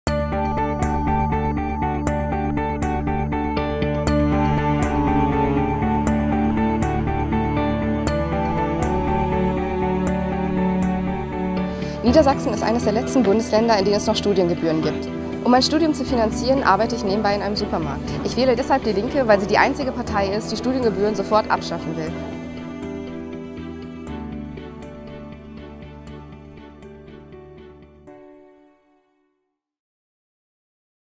Wir wählen links Studiengebühren jetzt abschaffen
Jessica Kaußen, 22, studiert Wirtschaftsinformatik an der Universität Hildesheim. In diesem Video erklärt sie, warum sie am 20. Januar DIE LINKE wählt.